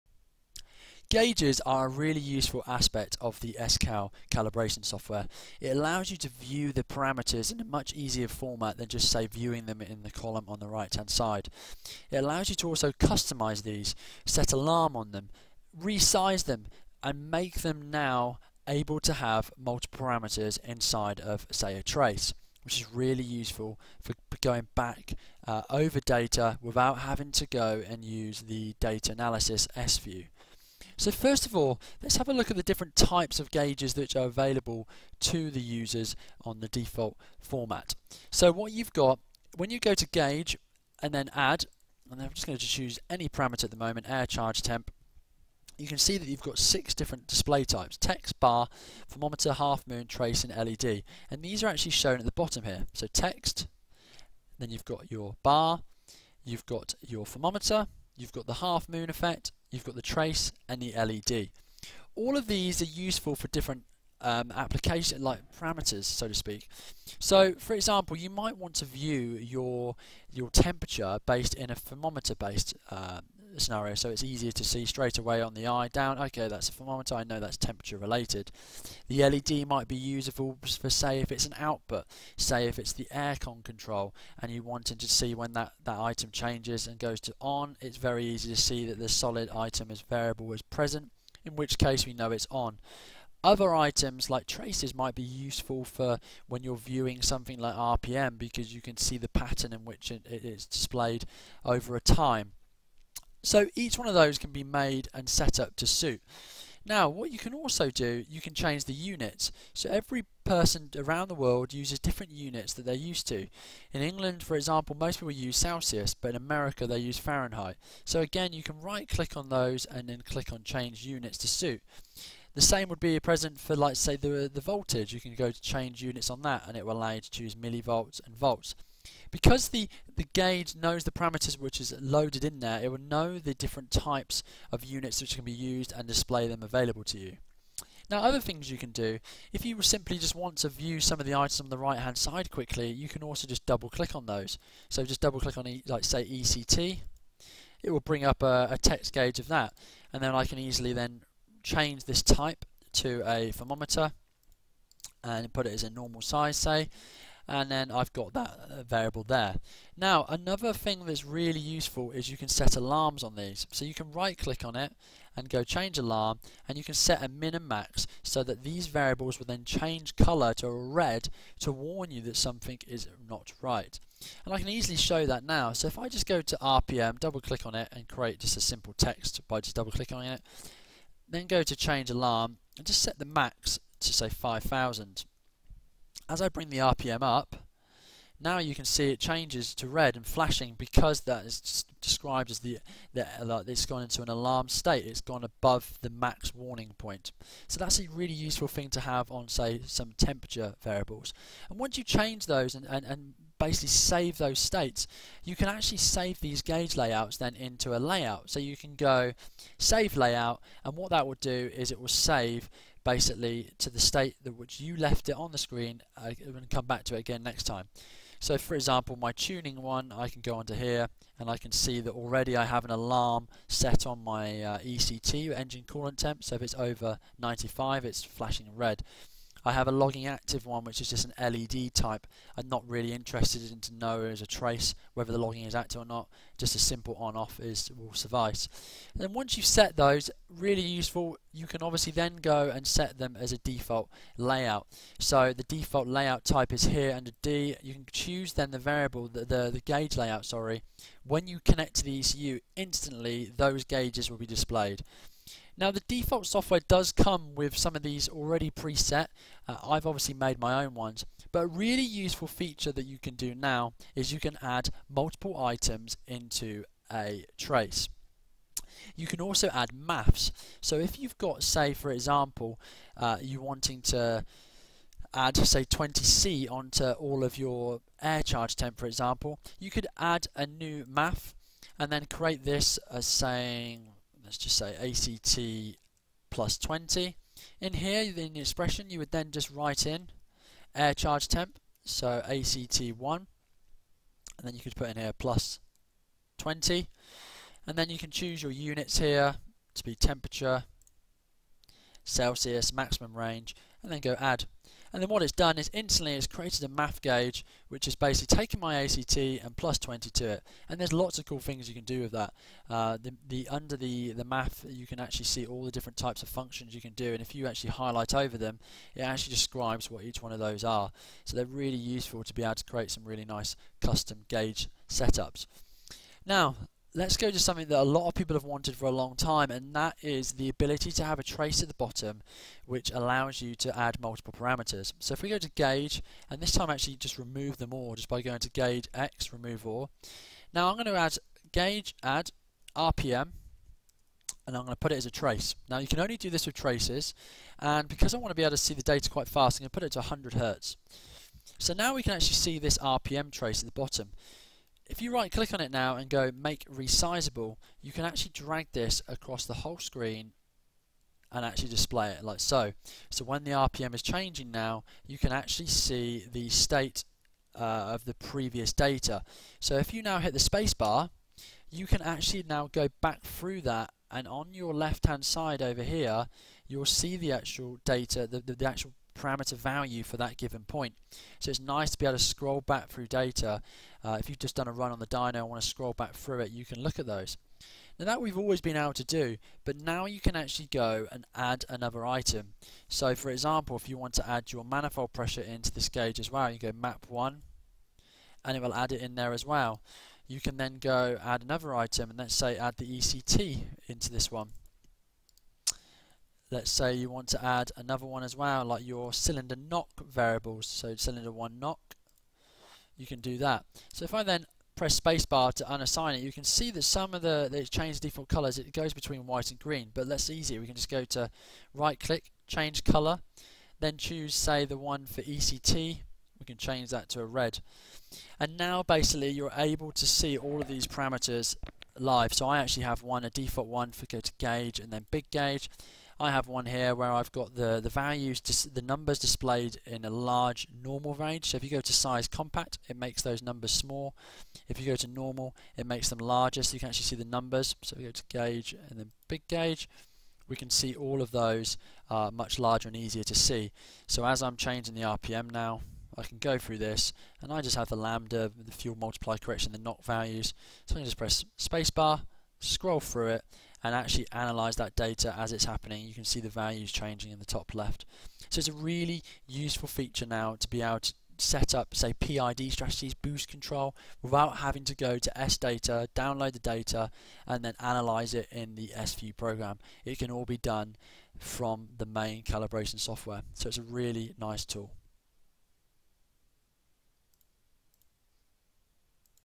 Syvecs | Scal - Gauge Setup
Explaining the different types of Gauges and what can be done with them